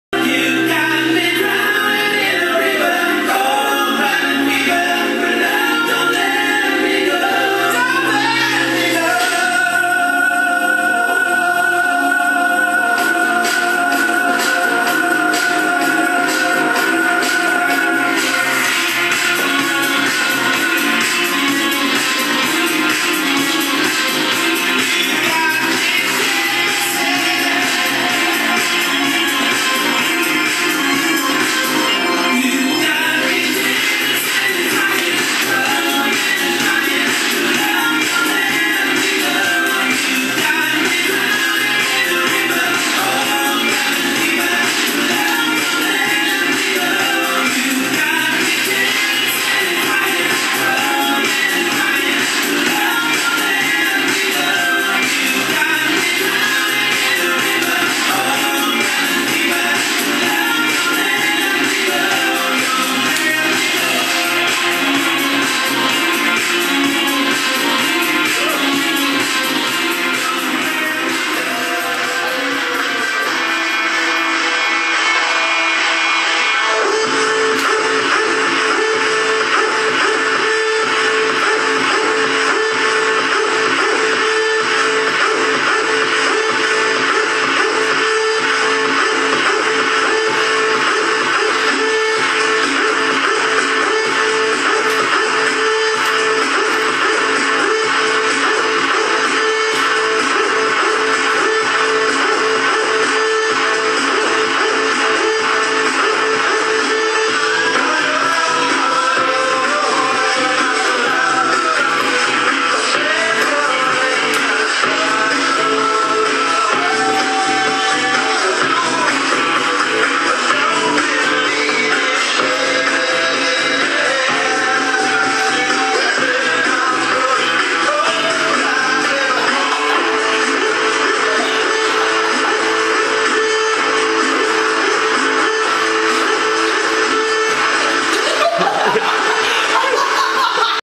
Liquidemotions.com davidnagOoyen rav'n light show
OK I lied... The gloves that I am using are a mix of kiwi and flash back lights... Enjoy!

These gloves can be purchased at liquidemotions.com